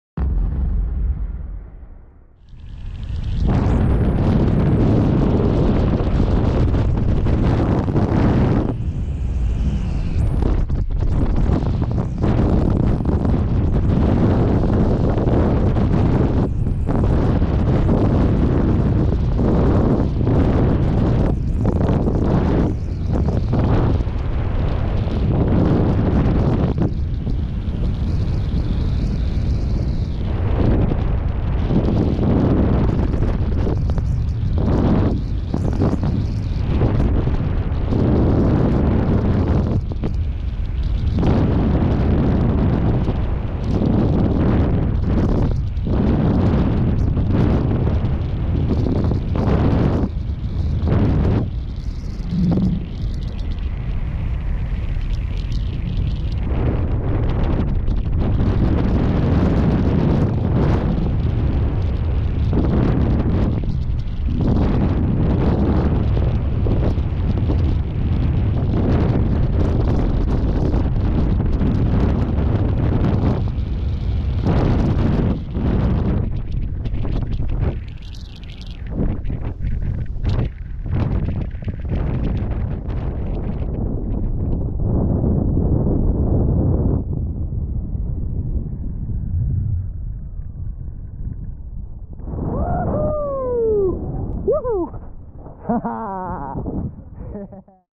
GoPro Awards: Rocky New Zealand Speedfly in 4K
Million Dollar Challenge recipient, Angus Sellen, sends it in 360 down Shark's Tooth Mountain, New Zealand with his GoPro MAX and HERO8 Black.